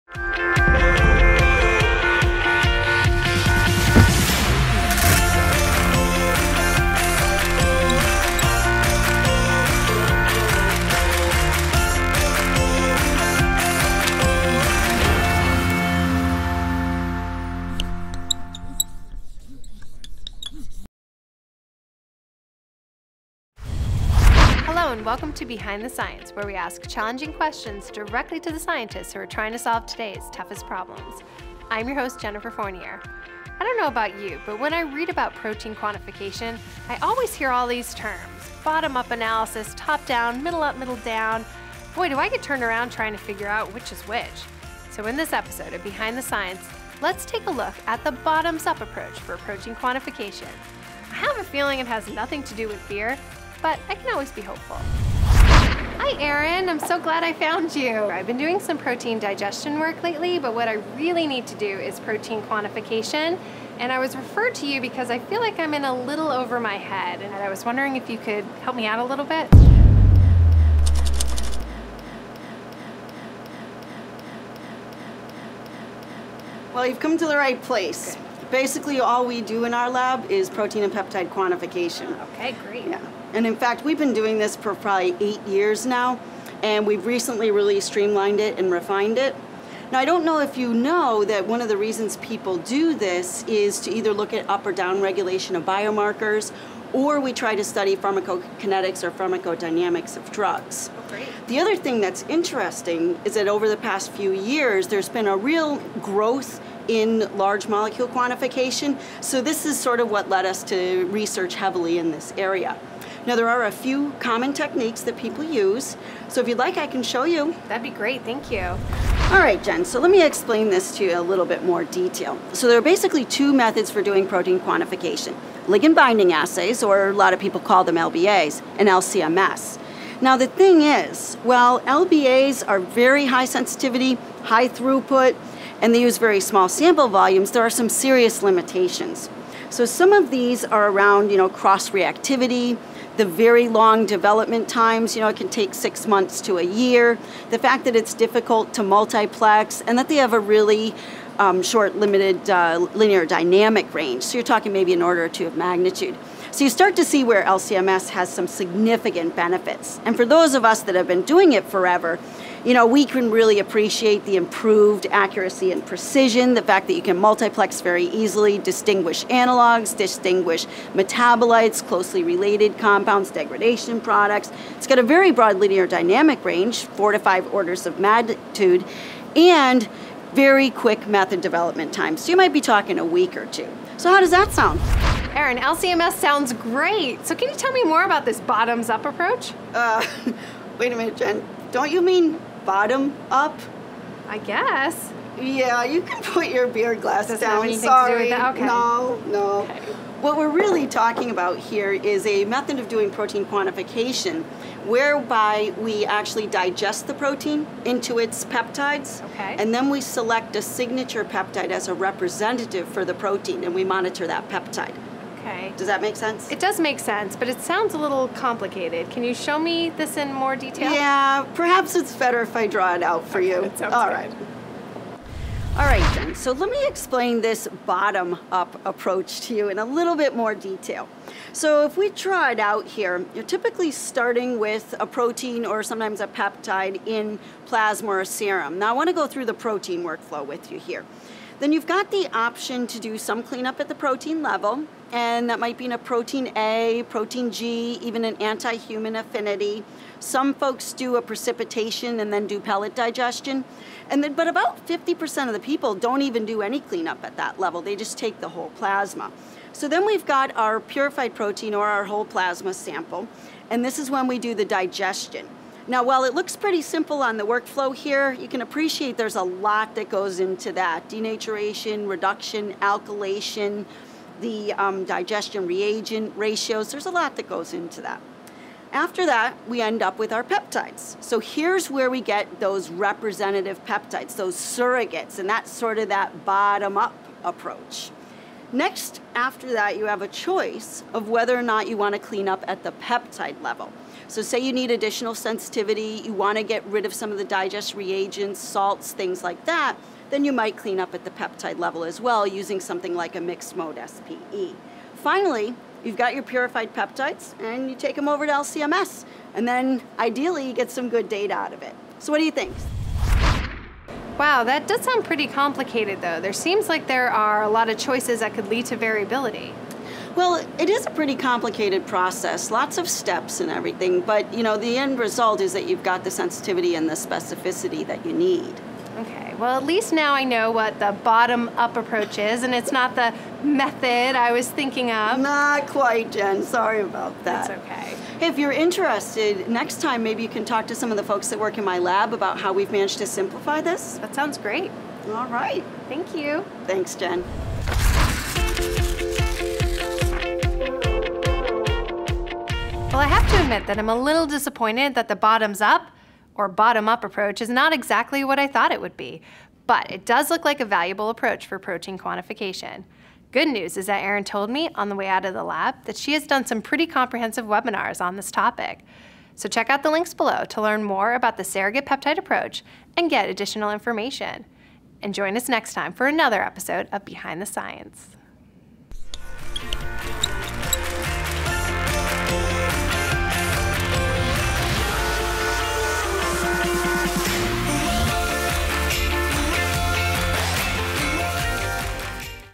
Behind the Science, ep. 8: "Bottoms" up
Protein quantification: bottoms up or bottom-up? As our host Jen Fournier learns, LC-MS/MS is a good alternative to ligand binding assays, and Waters scientists have been developing methods and technologies to support protein quantification for more than 8 years. Erin Chambers, principal scientist, shows Jen how to perform a surrogate peptide approach, where a protein is digested into its peptide, and a signature peptide is selected to monitor as a representative in a quantitative assay. No beer mugs in the lab!